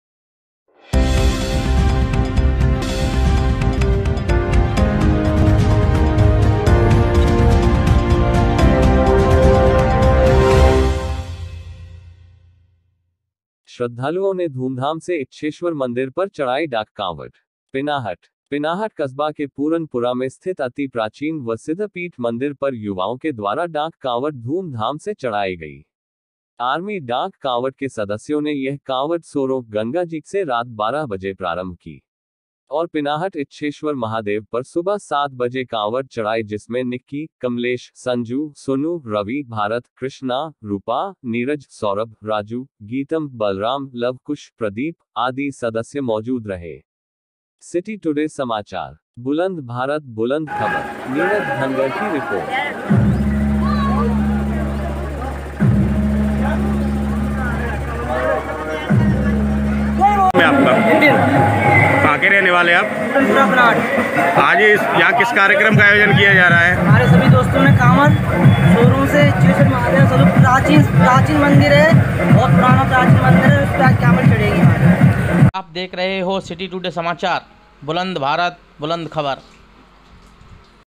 श्रद्धालुओं ने धूमधाम से इच्छेश्वर मंदिर पर चढ़ाई डाक काँवड।
पिनाहट ।  पिनाहट कस्बा के पूरन पुरा में स्थित अति प्राचीन व सिद्ध पीठ मंदिर पर युवाओं के द्वारा डांक काँवड धूम धाम से चढ़ाई गई। आर्मी डांक काँवड के सदस्यों ने यह काँवड सोरों ( गंगा जी) से रात बारह बजे प्रारंभ की
और पिनाहट  इच्छेश्वर महादेव पर सुबह 7 बजे काँवड चढ़ाई जिसमें निक्की, कमलेश, संजू, सोनू, रवि, भारत, कृष्णा, रूपा, नीरज, सौरभ, राजू, गीतम, बलराम, लवकुश, प्रदीप, आदि सदस्य मौजूद रहे। सिटी टुडे समाचार। बुलंद भारत बुलंद खबर। नीरज धनगर की रिपोर्ट